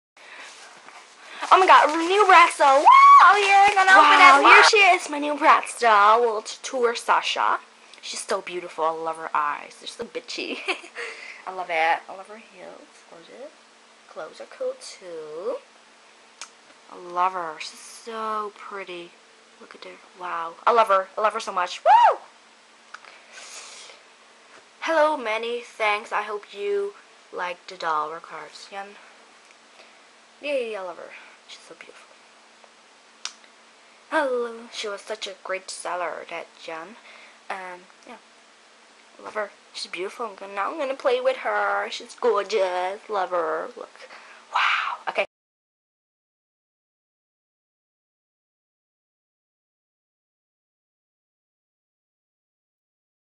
My New BratzDoll Once Again =D
Yeah i got again a new bratzdoll, World Tour sasha from ebay again =D her name will be Bolayna =} i love her so much =D